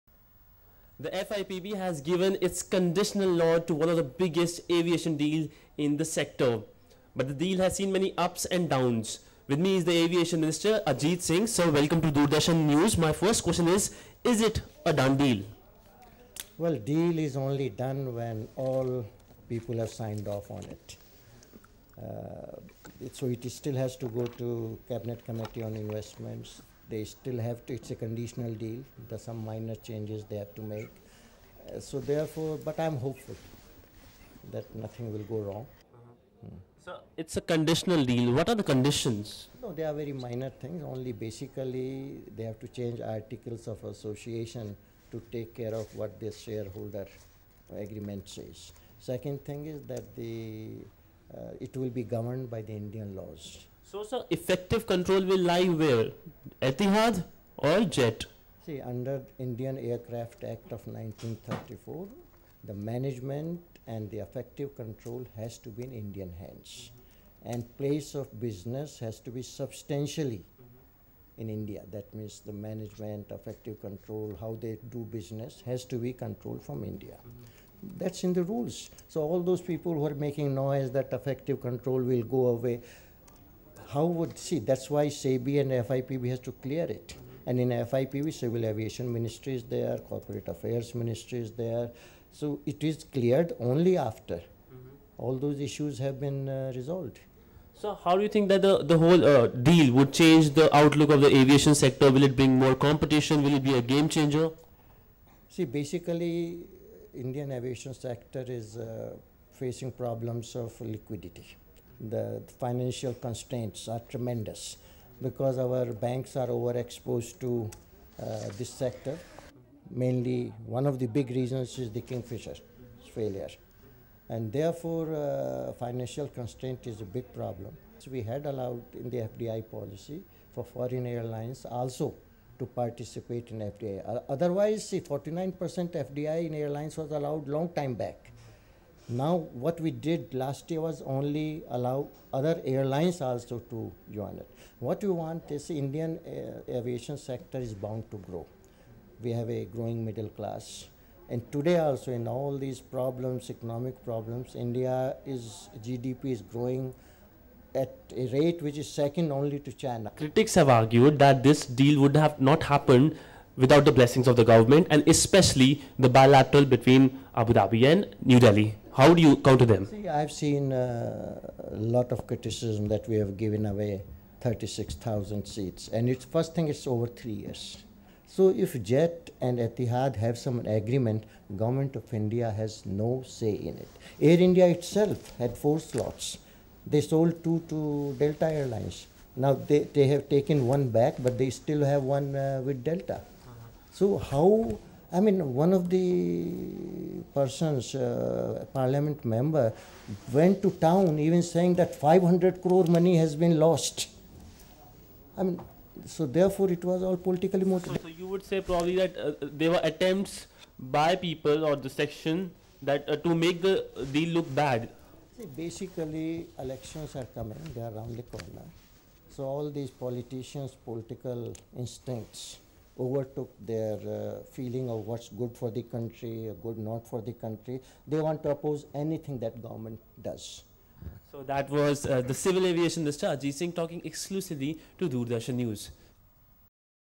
FIPB Gives Nod to Jet-Etihad deal
FIPB Gives Nod to Jet-Etihad deal; Civil Aviation Minister Ajit Singh Talks to DD NEWS